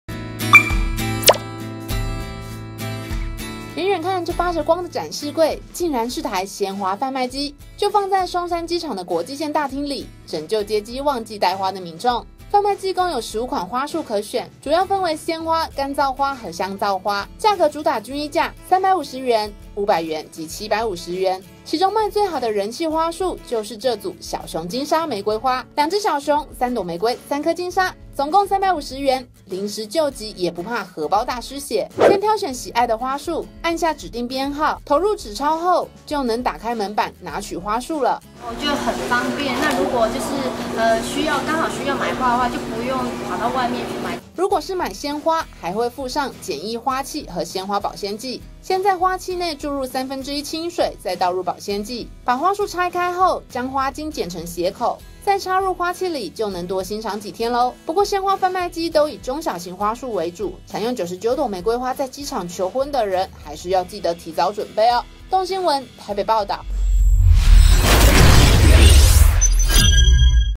機場接機沒帶花？這台機器神救援 | 台灣蘋果日報
松山機場大廳裡一台發著光的展示櫃，是台灣機場第1台鮮花販賣機，供接機忘記帶花的民眾救急用。鮮花販賣機共有15款花束可選，主要分為鮮花、乾燥花和香皂花，價格主打均一價350元、500元及750元。